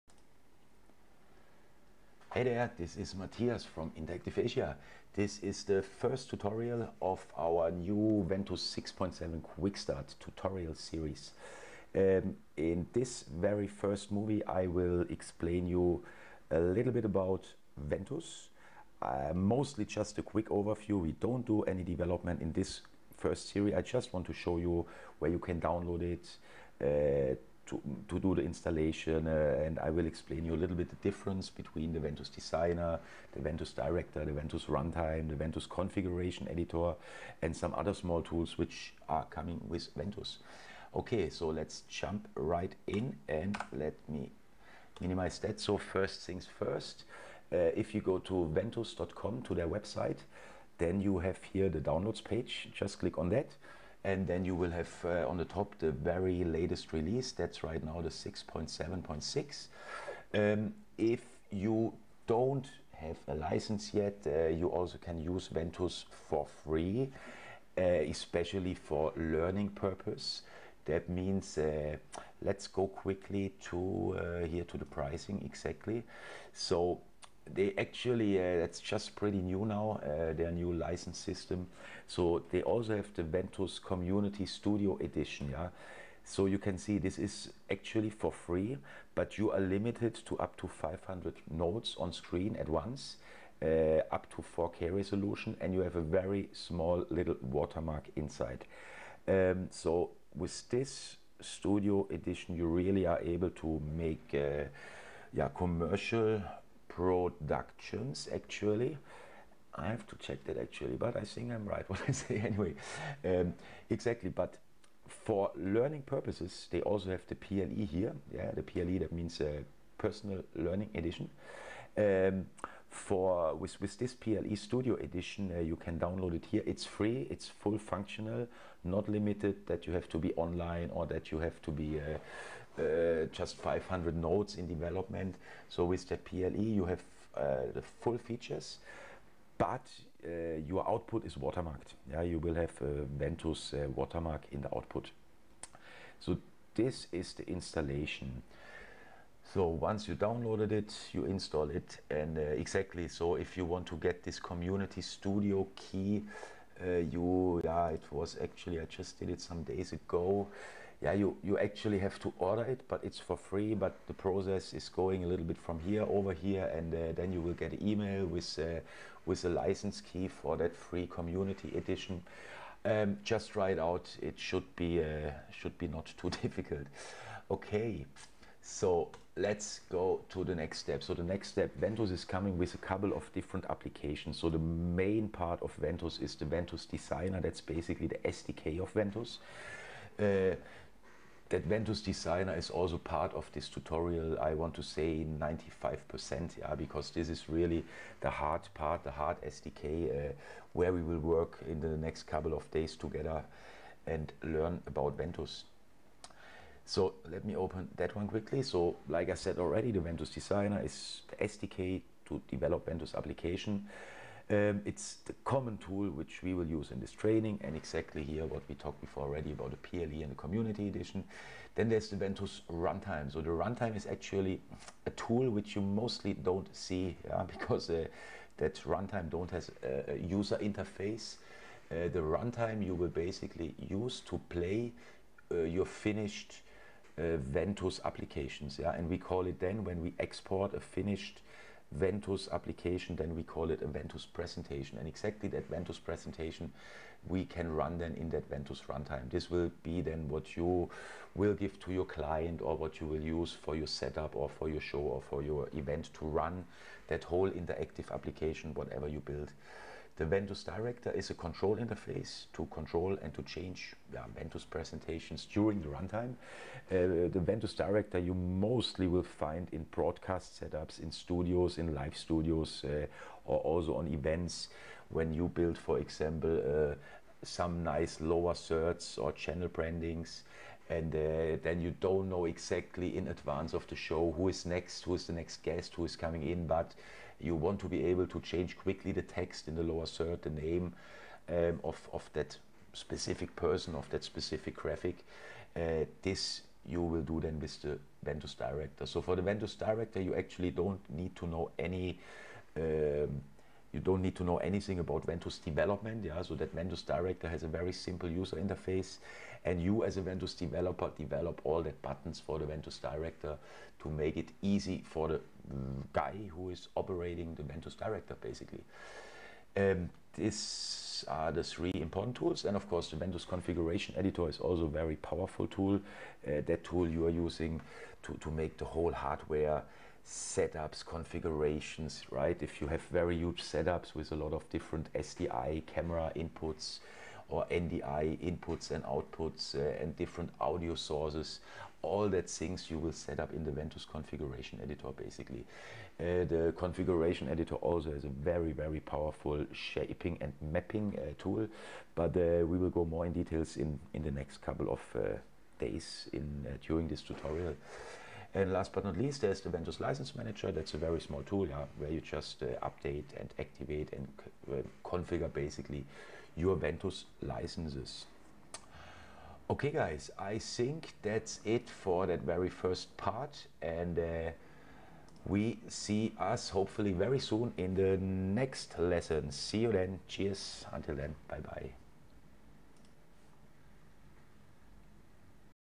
Ventuz Quick Start Tutorial - 01 - Introduction
This is a Ventuz for beginners tutorial. I use Ventuz since close to 10 years now and we are also using Ventuz since many years with our company Interactive Asia.